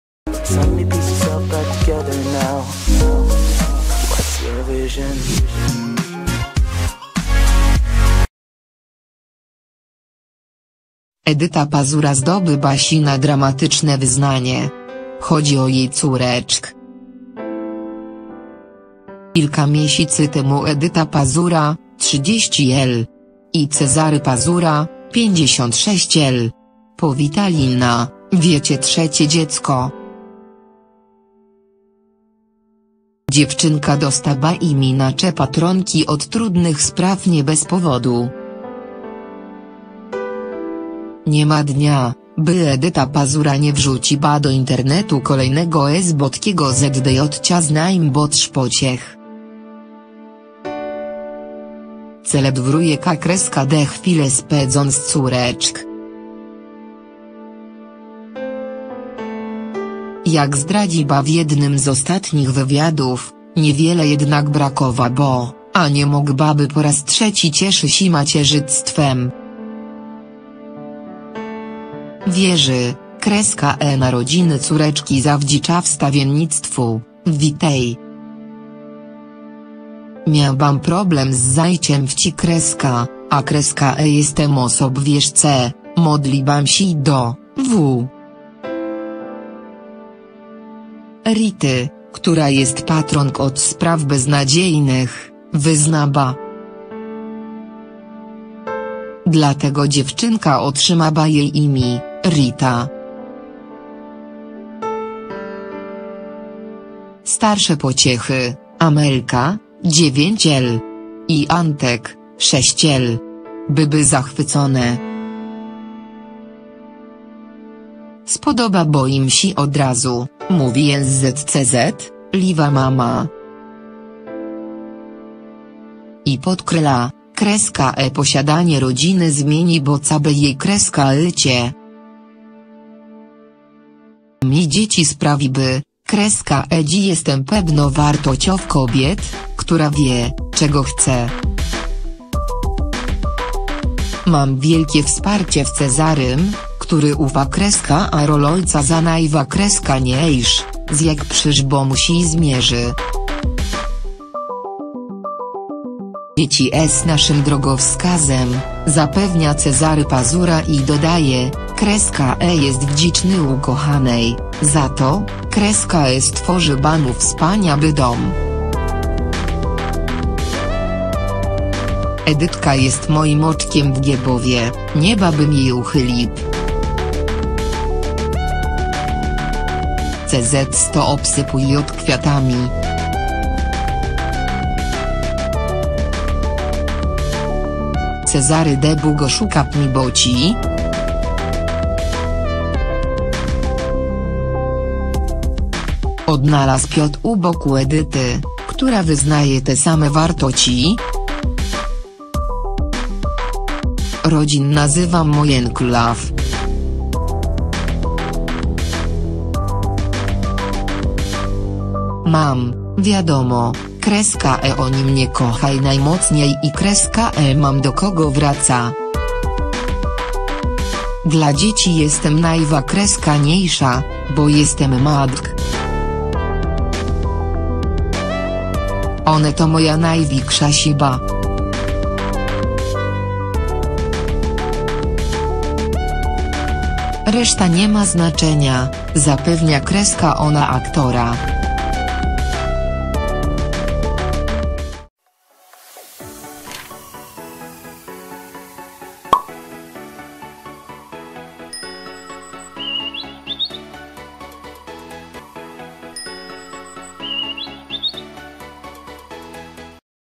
Edyta Pazura zdobyła się na dramatyczne wyznanie! Chodzi o jej córeczkę!
Edyta Pazura zdobyła się na dramatyczne wyznanie! Chodzi o jej córeczkę! - Kilka miesięcy temu Edyta Pazura (30 l.) i Cezary Pazura (56 l.) powitali na świecie trzecie dziecko. Dziewczynka dostała imię na cześć patronki od trudnych spraw nie bez powodu.
Gossip o gwiazdach ze świata p